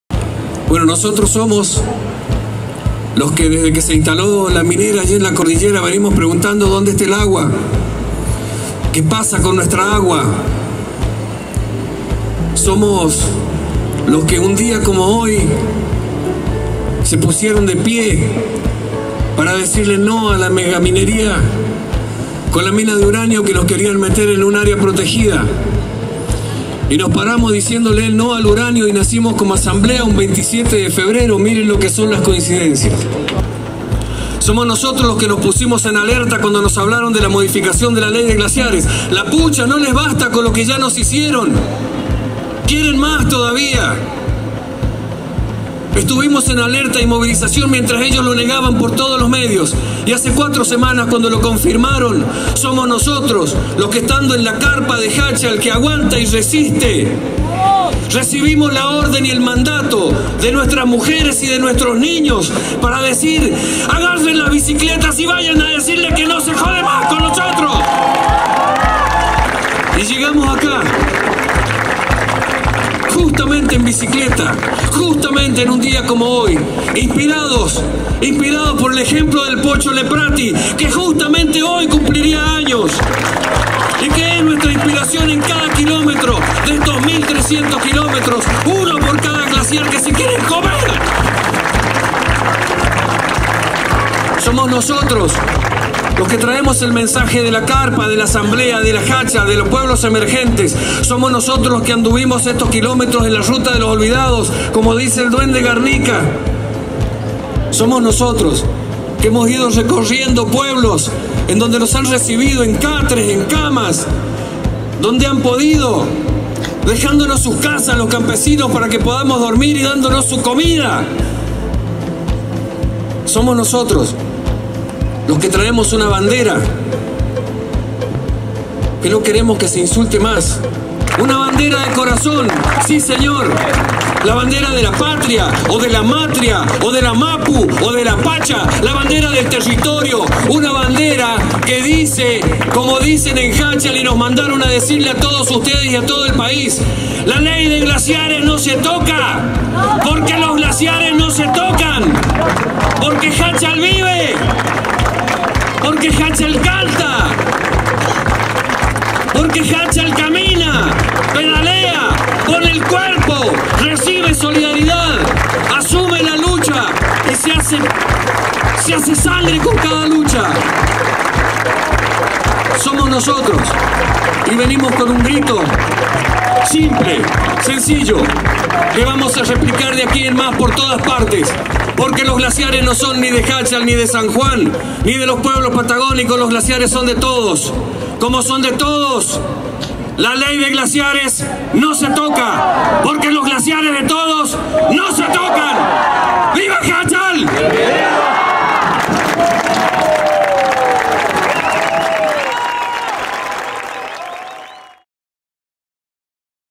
Somos nosotros en defensa de la Ley de Glaciares
Llegada a Buenos Aires de la bicicleteada en defensa de los glaciares y por la plena aplicación de la Ley 25.639 de presupuestos mínimos de protección de glaciares y ambiente periglaciar. Los ciclistas de Jáchal, Provincia de San Juan emprendieron un viaje que atravezó cuatro provincias en trayecto de mas de 1300 Km. Aclamados en cada pueblo y ciudad, que fueron uniendo,  la gesta se tranformó en una gran fiesta popular. Fueron recibidos en Bs As por muchas organizaciones y celebrada por gran cantidad de personas.